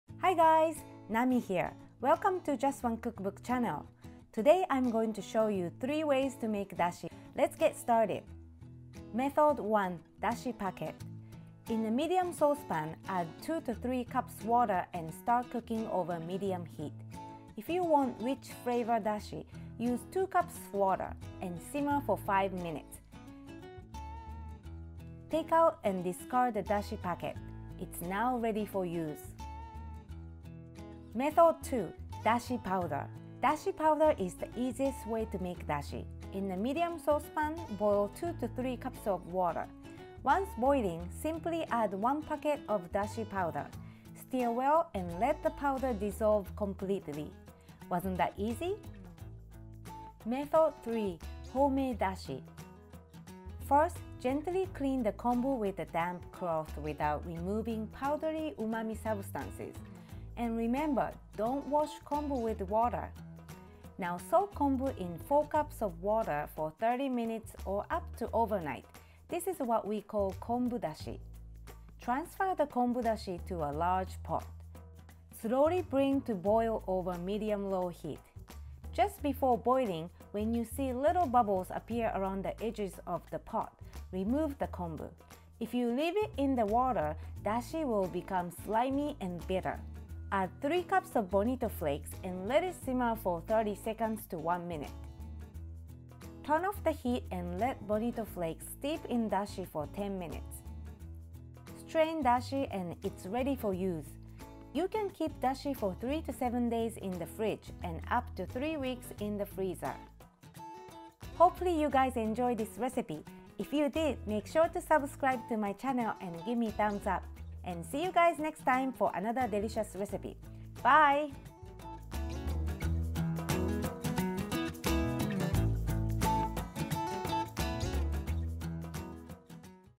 How To Make Dashi 3 Ways (Recipe) だしの作り方３種類（レシピ）
You can easily make dashi (Japanese stock) for Japanese dishes at home and I’ll show you 3 ways to make it.

Ingredients:

For Dashi Packet & Dashi Powder
2-3 cups water
1 dashi packet or 1 pack dashi powder (3 g/1 tsp.)

For Homemade Dashi
4x5" (20 g) kombu
4 cups water
3 cups (30 g) bonito flakes (katsuobushi)

★ INGREDIENTS & COOKING EQUIPMENT ★

DASHI PACKET
DASHI POWDER
BONITO FLAKES (KATSUOBUSHI)
SAUCEPAN 1
SAUCEPAN 2

★ MUSIC ★

★ VIDEO EQUIPMENT ★